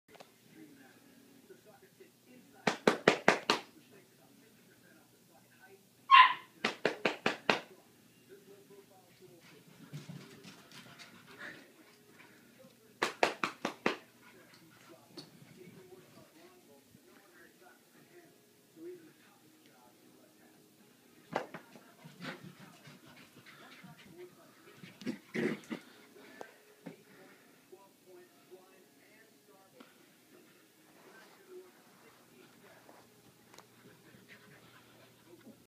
Psych Dog goin nuts.
Libby goes nuts every night around 930.